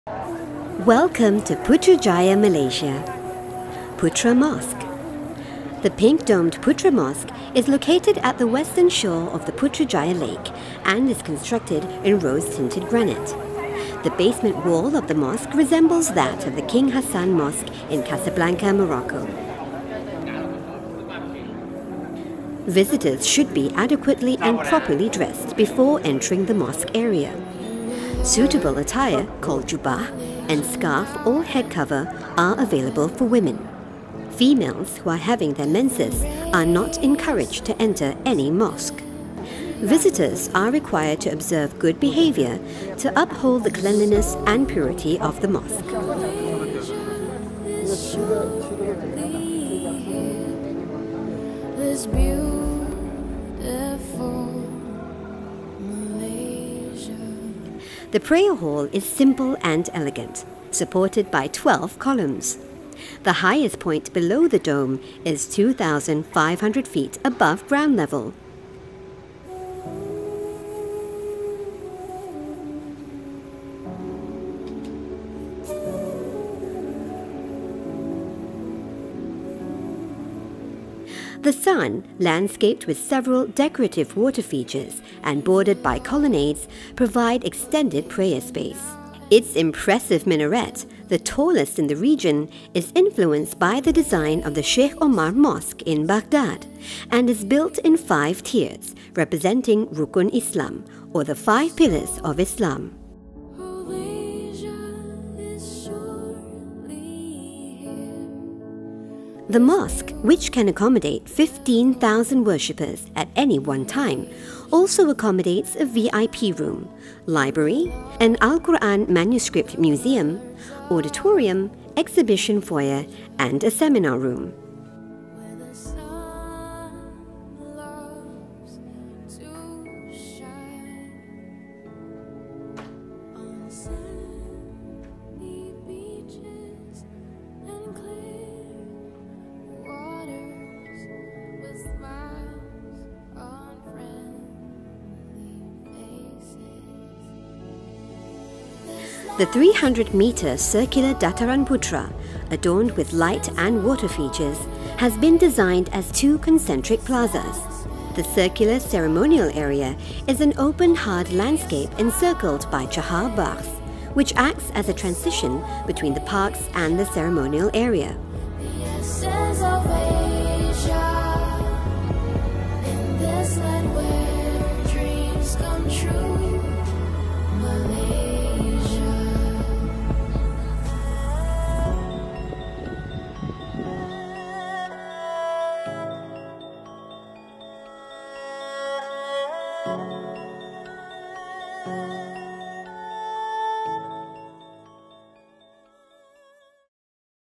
Discover Putrajaya in 360° - Putra Mosque Virtual Tour
One of the first Putrajaya structures to be completed was the Putra Mosque which, till today, remains an important landmark of Putrajaya. Its dusky pink dome, topped with a gold tiled final measuring 76 meters above ground level, has been the point of reference for many who navigate the roads around the city. Surprisingly, the elaborately-decorated dome took only six weeks to complete due to the use of modern technology which was able to create a perfect mold of the dome. Pre-fabricated sections of the dome were made off-site and mounted on the mosque without the use of scaffolding and in 30 percent less time than conventional methods.

Today, the mosque, which seems to “float” on the Putrajaya Lake, welcomes as many as 10,000 worshippers in its vast prayer hall.